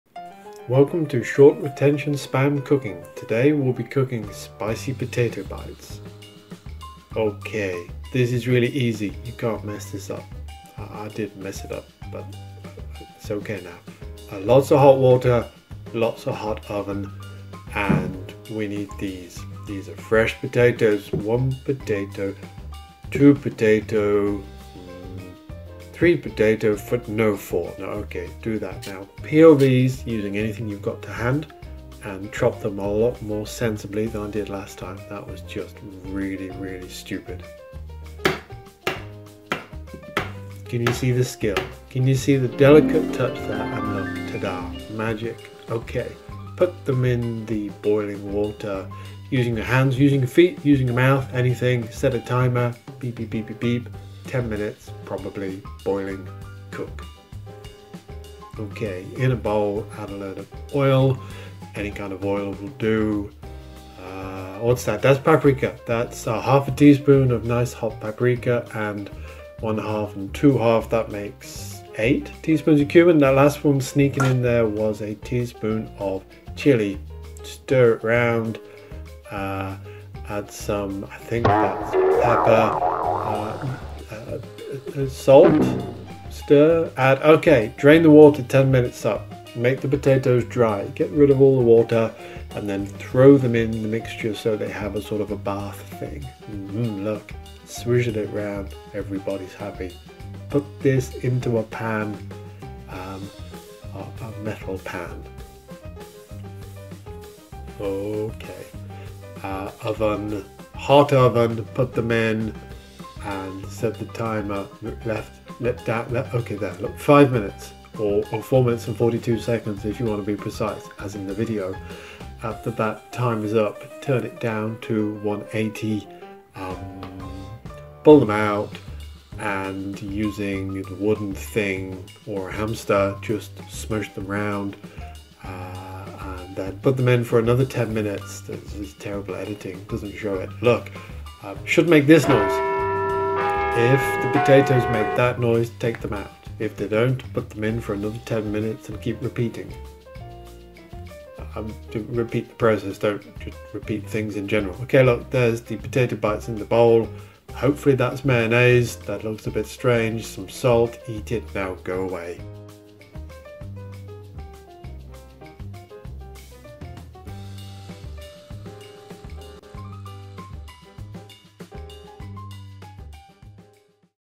Spicy Potato Bites - Short Attention Span Cooking
Spicy Potato Bites

Welcome to another Short Attention Span Cooking video! We are the only comedy cooking channel with 4k videos that are based in New Zealand.

Today's recipe in full:

Enough for 2 or 3 people

1. Peel and chop 3 medium size potatoes into bite-sized chunk
2. Boil for 10 mins in salted water, drain and let sit in the hot pan to dry.
3. Mix 2 tablespoons of cooking oil, 1/2 tsp of good paprika, 1 tsp of ground cumin and 1/2 tsp of chili powder.
4. Stir the potatoes into the oil.
5. Put a pan into a high/hot oven for a couple of minutes, remove and add potato mixture and a splash of oil.
5. Cook on high for 5 mins then reduce heat to 180C/350F.
6. Stir potatoes in the pan to ensure they don't stick then put back in the oven for 10 mins.
6. Repeat this process until the potatoes are crunchy on the outside.

Sorry about the green line at the bottom of the video, it's not there on purpose. It follows me around. I can see a green line now in fact, and I have my eyes closed. Maybe I need help.